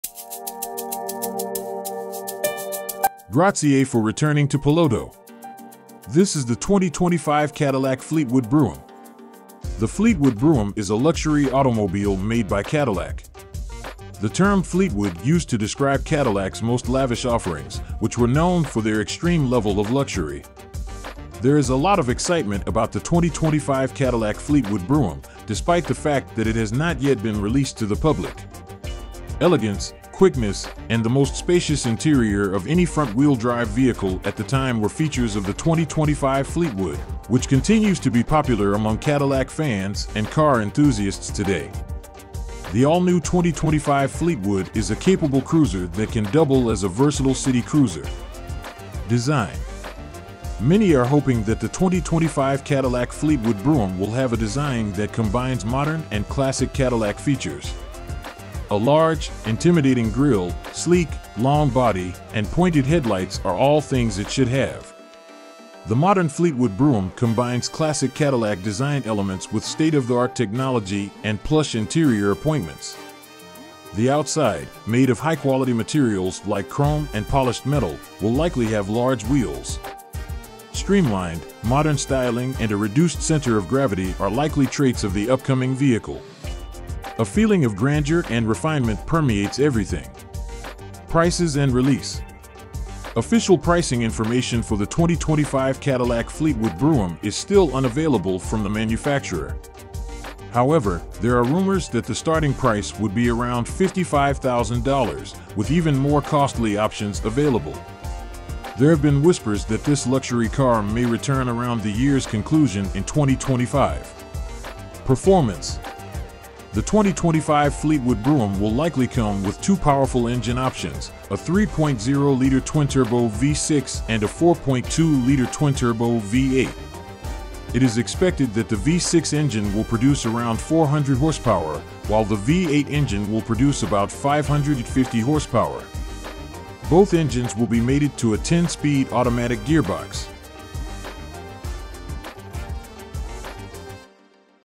2025 Cadillac Fleetwood Brougham Unveiled - combining modern and classic Cadillac luxury!
The Fleetwood Brougham is a luxury automobile made by Cadillac. The term "Fleetwood" used to describe Cadillacs' most lavish offerings, which were known for their extreme level of luxury. There is a lot of excitement about the 2025 Cadillac Fleetwood Brougham, despite the fact that it has not yet been released to the public. Elegance, quickness, and the most spacious interior of any front-wheel-drive vehicle at the time were features of the 2025 Fleetwood, which continues to be popular among Cadillac fans and car enthusiasts today. The all-new 2025 Fleetwood is a capable cruiser that can double as a versatile city cruiser. Many are hoping that the 2025 Cadillac Fleetwood Brougham will have a design that combines modern and classic Cadillac features. A large, intimidating grille, sleek, long body, and pointed headlights are all things it should have. The modern Fleetwood Brougham combines classic Cadillac design elements with state-of-the-art technology and plush interior appointments. The outside, made of high-quality materials like chrome and polished metal, will likely have large wheels. Streamlined, modern styling and a reduced center of gravity are likely traits of the upcoming vehicle. A feeling of grandeur and refinement permeates everything. Official pricing information for the 2025 Cadillac Fleetwood Brougham is still unavailable from the manufacturer. However, there are rumors that the starting price would be around $55,000, with even more costly options available. There have been whispers that this luxury car may return around the year's conclusion in 2025. The 2025 Fleetwood Brougham will likely come with two powerful engine options: a 3.0 liter twin-turbo V6 and a 4.2 liter twin-turbo V8. It is expected that the V6 engine will produce around 400 horsepower, while the V8 engine will produce about 550 horsepower. Both engines will be mated to a 10-speed automatic gearbox.